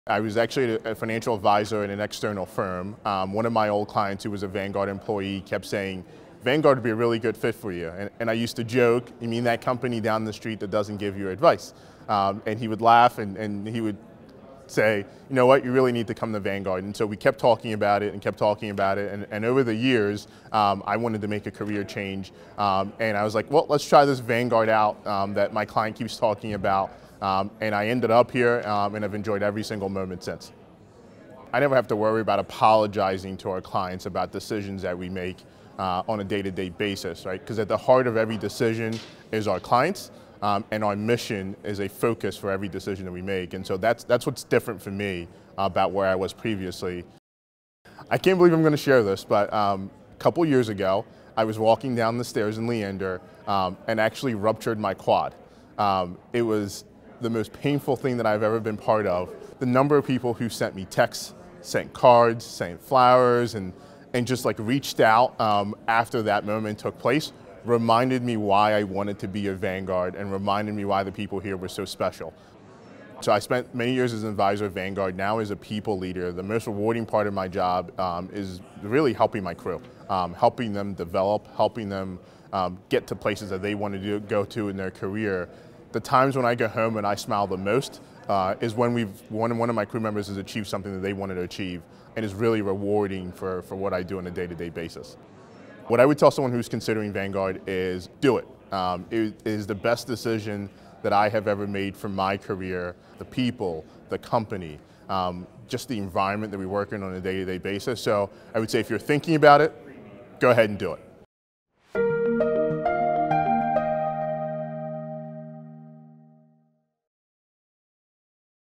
Meet Deji, Financial Advisor Manager
Meet Deji, a Financial Advisor Manager who shares how he found the right fit with Vanguard. Deji’s strong client focus and passion for developing his crew have remained a priority throughout his career.